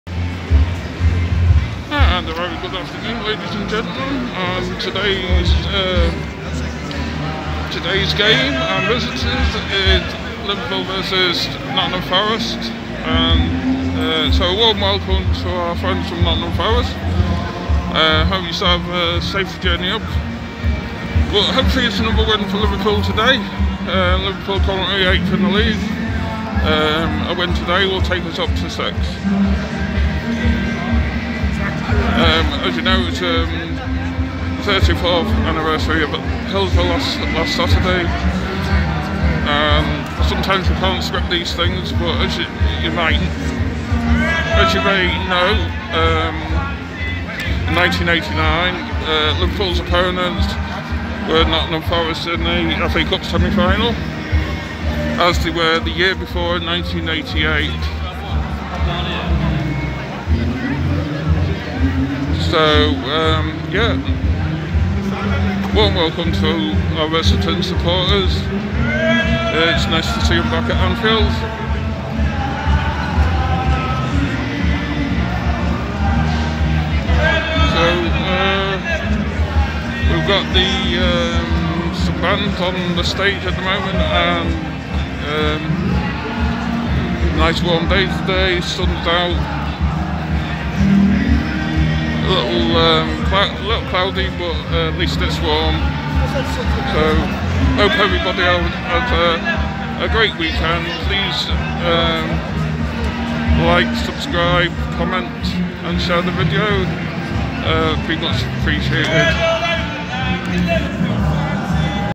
Liverpoolfc vs Nottingham forest welcome back to Anfield YNWA
A warm welcome back to anfield to all the nottingham forest supporters. thank you for your support YNWA